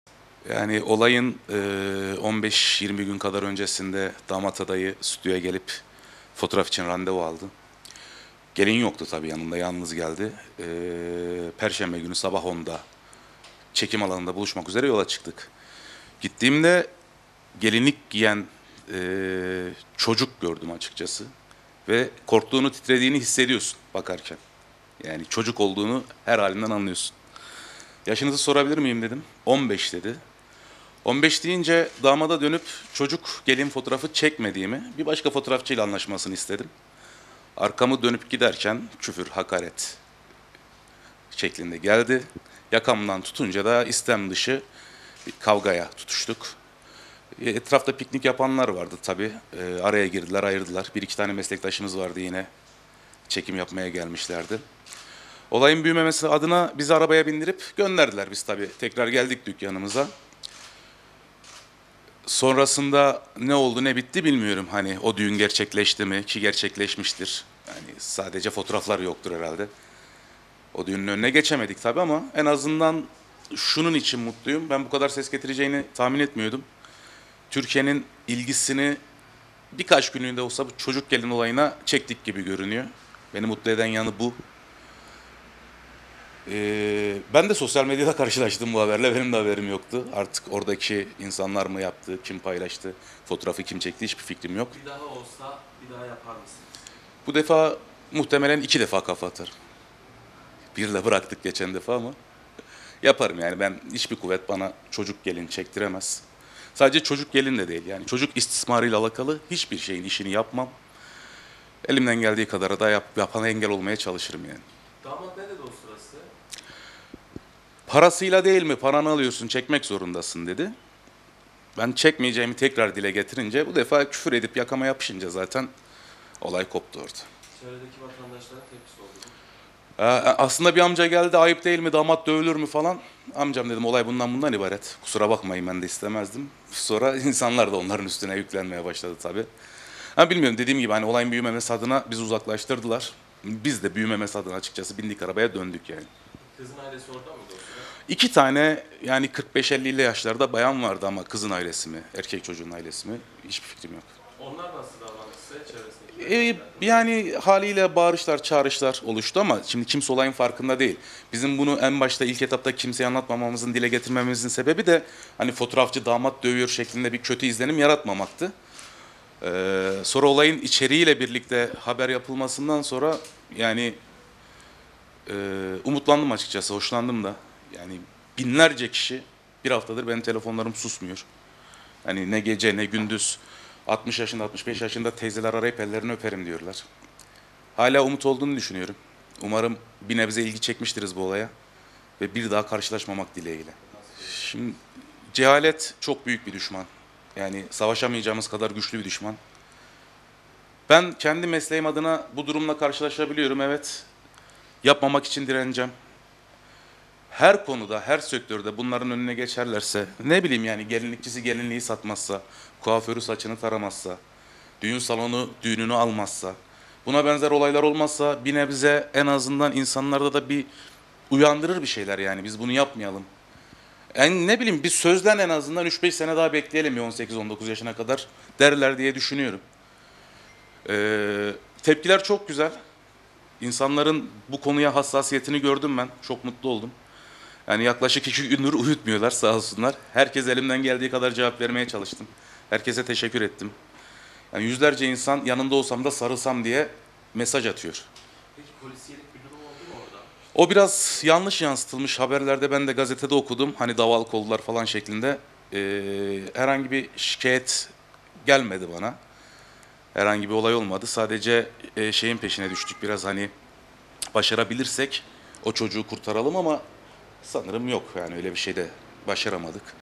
Düğün fotoğrafçısı gelini görünce damadı dövdü
Malatya'da düğün fotoğrafını çekeceği gelinin yaşının 15 olduğunu öğrenince tepki gösterip çekip yapmayacağını belirten fotoğrafçı Onur Albayrak, damatla tartışıp kavga etti. Damadın burnu kırılırken, aynı durumla karşılaşması halinde daha ağır bir tepki vereceğini belirten Albayrak, "Hiçbir kuvvet bana çocuk gelin çektiremez" dedi.